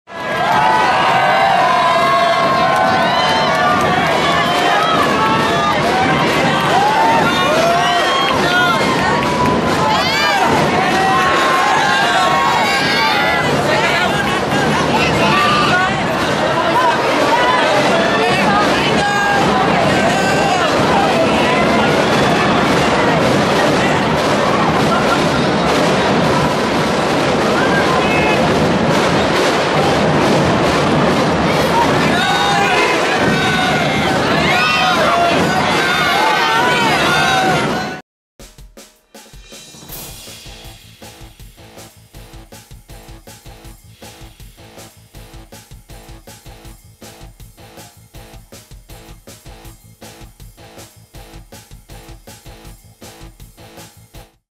UST crowd hails Ferrer, Daquioag, Vigil
Watch UST Growling Tigers Highlights with Ferrer,Daquioag and Vigil